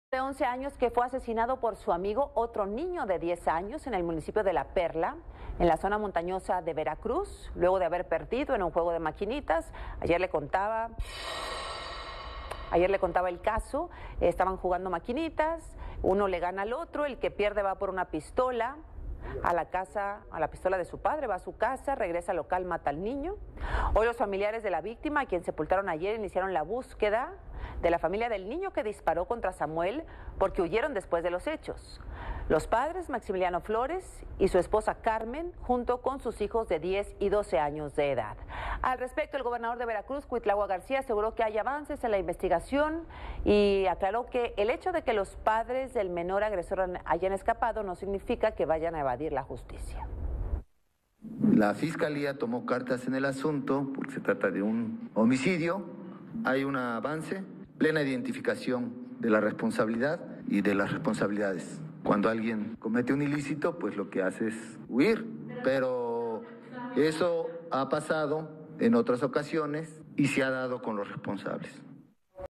Continúa la búsqueda del menor que disparó en contra de Samuel
AzucenaALas10 | Sobre Samuel, el niño de 11 años que fue asesinado por su amigo, de 10 años, en La Perla, Veracruz, sus familiares iniciaron la búsqueda de la familia del niño que le disparó

Cuitláhuac García aseguró que la fiscalía ya tomó cartas en el asunto